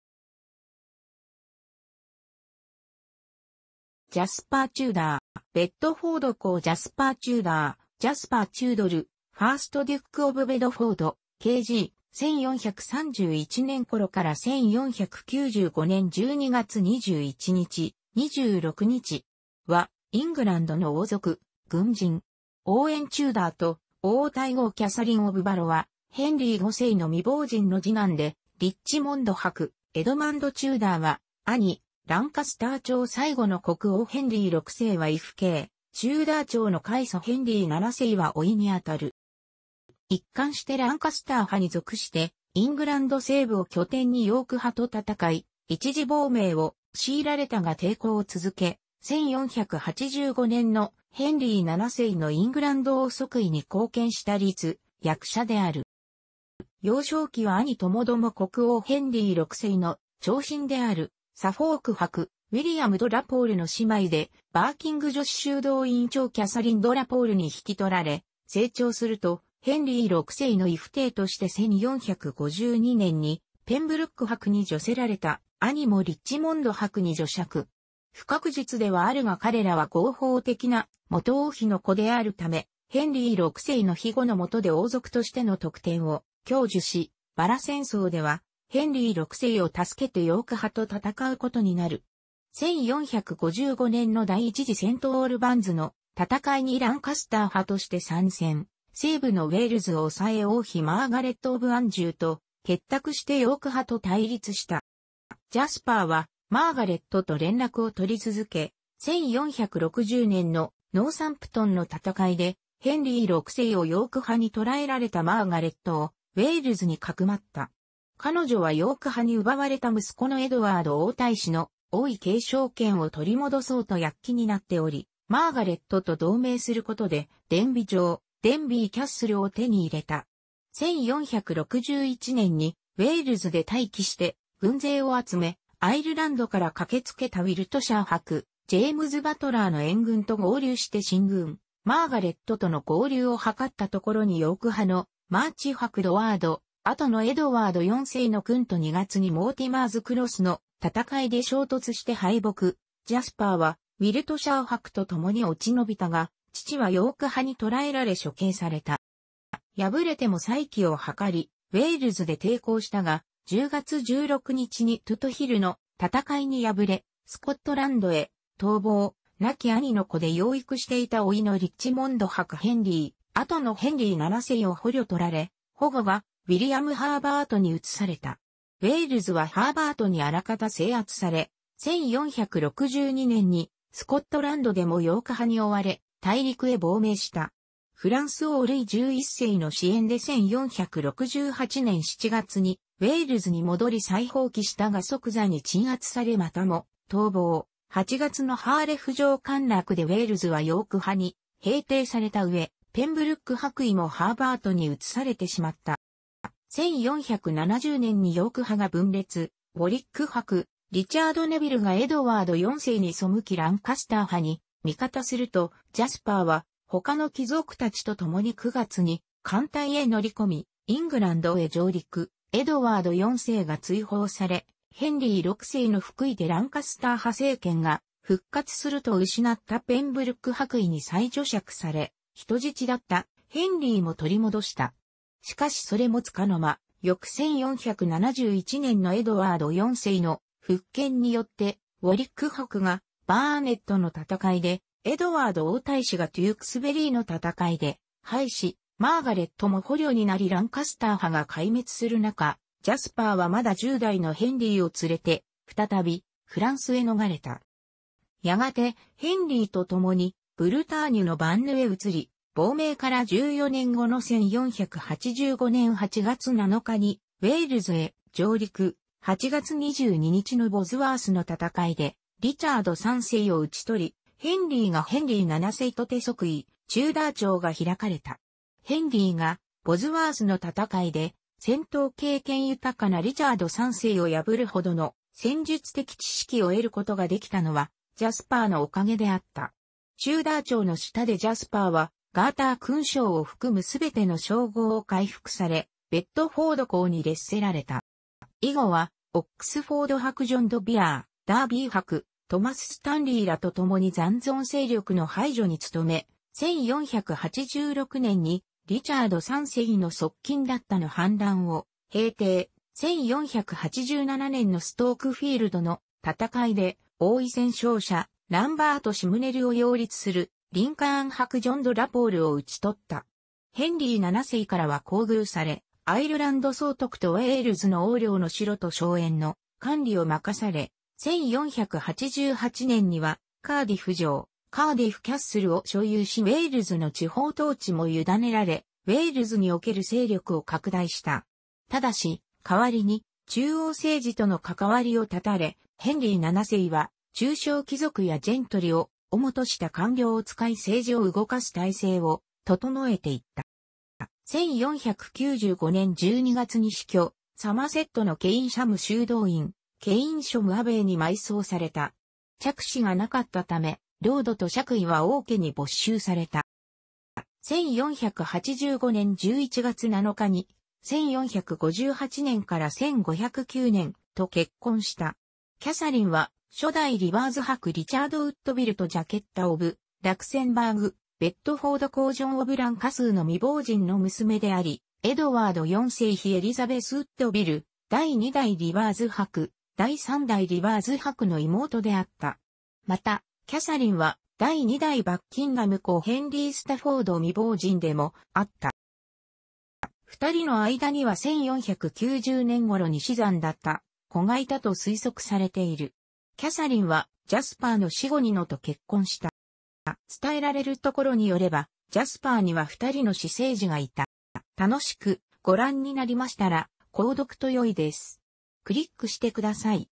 ジャスパー・テューダー (ベッドフォード公)
アイルランド総督_(ロード・レフテナント)

ベッドフォード公ジャスパー・テューダー（Jasper Tudor, 1st Duke of Bedford, KG, 1431年頃 - 1495年12月21日（26日?））は、イングランドの王族・軍人。オウエン・テューダーと王太后キャサリン・オブ・ヴァロワ（ヘンリー5世の未亡人）の次男でリッチモンド伯エドマンド・テューダーは兄、ランカスター朝最後の国王ヘンリー6世は異父兄、テューダー朝の開祖ヘンリー7世は甥に当たる。

一貫してランカスター派に属してイングランド西部を拠点にヨーク派と戦い、一時亡命を強いられたが抵抗を続け、1485年のヘンリー7世のイングランド王即位に貢献した立役者である。

幼少期は兄共々国王ヘンリー6世の寵臣であるサフォーク伯ウィリアム・ド・ラ・ポールの姉妹でバーキング女子修道院長キャサリン・ド・ラ・ポールに引き取られ、成長するとヘンリー6世の異父弟として1452年にペンブルック伯に叙せられた（兄もリッチモンド伯に叙爵）。不確実ではあるが彼らは合法的な「元王妃の子」であるため、ヘンリー6世の庇護の下で王族としての特典を享受し、薔薇戦争ではヘンリー6世を助けてヨーク派と戦うことになる。1455年の第一次セント・オールバンズの戦いにランカスター派として参戦、西部のウェールズを抑え王妃マーガレット・オブ・アンジューと結託してヨーク派と対立した。

ジャスパーはマーガレットと連絡を取り続け、1460年のノーサンプトンの戦いでヘンリー6世をヨーク派に捕らえられたマーガレットをウェールズに匿った。彼女はヨーク派に奪われた息子のエドワード王太子の王位継承権を取り戻そうと躍起になっており、マーガレットと同盟することでデンビ城（Denbigh Castle）を手に入れた。1461年にウェールズで待機して軍勢を集め、アイルランドから駆け付けたウィルトシャー伯ジェームズ・バトラーの援軍と合流して進軍、マーガレットとの合流を図った所にヨーク派のマーチ伯エドワード（後のエドワード4世）の軍と2月にモーティマーズ・クロスの戦いで衝突して敗北、ジャスパーはウィルトシャー伯と共に落ち延びたが、父はヨーク派に捕らえられ処刑された。

敗れても再起を図りウェールズで抵抗したが、10月16日にトゥト・ヒルの戦いに敗れスコットランドへ逃亡、亡き兄の子で養育していた甥のリッチモンド伯ヘンリー（後のヘンリー7世）を捕虜に取られ、保護がウィリアム・ハーバートに移された。ウェールズはハーバートにあらかた制圧され、1462年にスコットランドでもヨーク派に追われ大陸へ亡命した。フランス王ルイ11世の支援で1468年7月にウェールズに戻り再蜂起したが即座に鎮圧されまたも逃亡、8月のハーレフ城陥落でウェールズはヨーク派に平定された上、ペンブルック伯位もハーバートに移されてしまった。

1470年にヨーク派が分裂、ウォリック伯リチャード・ネヴィルがエドワード4世に背きランカスター派に味方すると、ジャスパーは他の貴族達と共に9月に艦隊へ乗り込みイングランドへ上陸、エドワード4世が追放されヘンリー6世の復位でランカスター派政権が復活すると失ったペンブルック伯位に再叙爵され、人質だったヘンリーも取り戻した。しかしそれも束の間、翌1471年のエドワード4世の復権によってウォリック伯がバーネットの戦いで、エドワード王太子がテュークスベリーの戦いで敗死、マーガレットも捕虜になりランカスター派が壊滅する中、ジャスパーはまだ10代のヘンリーを連れて再びフランスへ逃れた。

やがてヘンリーと共にブルターニュのヴァンヌへ移り、亡命から14年後の1485年8月7日にウェールズへ上陸、8月22日のボズワースの戦いでリチャード3世を討ち取りヘンリーがヘンリー7世として即位、テューダー朝が開かれた。ヘンリーがボズワースの戦いで戦闘経験豊かなリチャード3世を破るほどの戦術的知識を得ることができたのは、ジャスパーのおかげであった。テューダー朝の下でジャスパーはガーター勲章を含む全ての称号を回復され、ベッドフォード公に列せられた。

以後はオックスフォード伯ジョン・ド・ヴィアー、ダービー伯トマス・スタンリーらと共に残存勢力の排除に努め、1486年にリチャー...